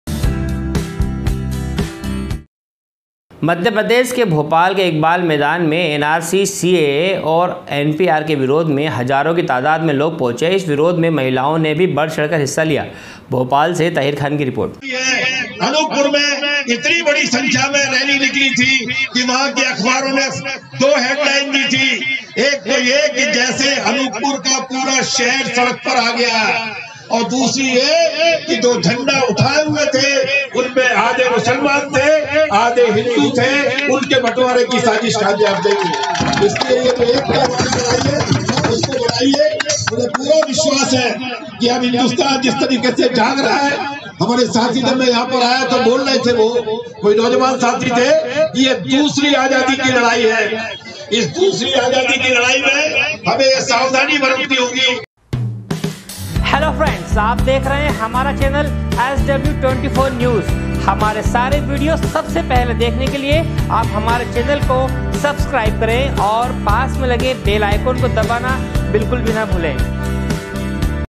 sw24news bhopal, 30.01.2020
मध्यप्रदेष के भोपाल इकबाल मैदान में एनआरसी सीएए आर एनपीआर के विरोध में हजारों की तादाद में लोग पहुंचे इस विरोध में महिलाओं ने भी बढ़चढ़कर हिस्सा लिया गया। भौपाल से ताहिर खान की रिपोर्ट